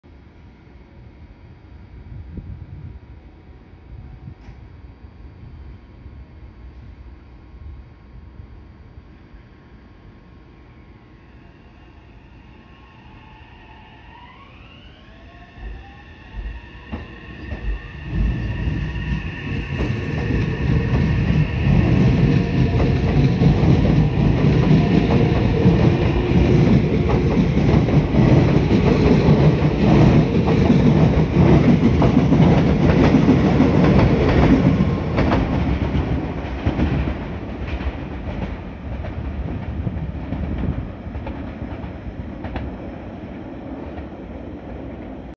●20221205 3150×2200系 発車 ＠東岡崎
撮影路線：名鉄 名古屋本線
撮影駅：東岡崎
行先：特急 名鉄岐阜
車両：3150×2200系
両数：2+6両＝8両
MH：ーー
備考欄：